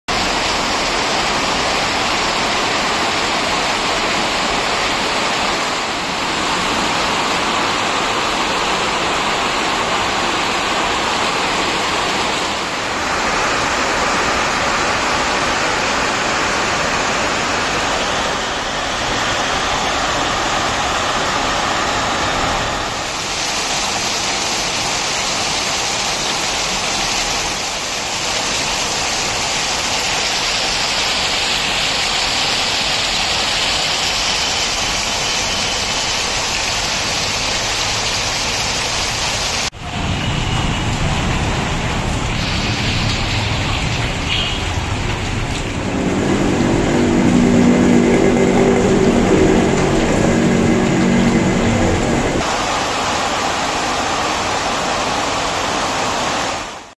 ഇതാണോ തീവ്ര മഴ.അതോ |ലോകാവസാനമോ Heavy Rain in Kochi Today
ഇതാണോ തീവ്ര മഴ.അതോ .ലോകാവസാനമോ ഇന്ന് രാവിലെ 1 മണിക്കൂർ കൊണ്ട് ഒരു വീടും പറമ്പും റോഡും കുളമായി . ലൈവ് ആയി കാണൂ